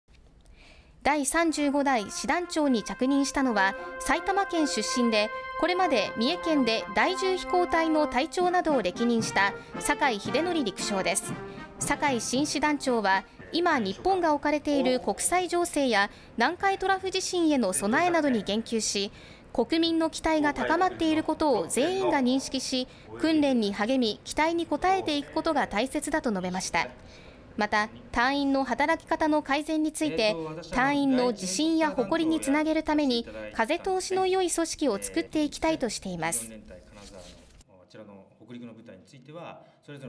【陸上自衛隊】新第１０師団長が着任 「部隊を強く 隊員を幸せに」
名古屋の陸上自衛隊・第１０師団に新師団長が着任し、「部隊を強く、隊員を幸せに」と目標を掲げました。

第３５代師団長に着任したのは、埼玉県出身でこれまで三重県で第１０飛行隊の隊長などを歴任した酒井秀典陸将（５５）です。

酒井新師団長はいま日本が置かれている国際情勢や、南海トラフ地震への備えなどに言及し、「国民の期待が高まっていることを全員が認識し、訓練に励み期待に応えていくことが大切だ」と述べました。

また、隊員の働き方の改善について、隊員の自信や誇りにつなげるために、「風通しのよい組織をつくっていきたい」としています。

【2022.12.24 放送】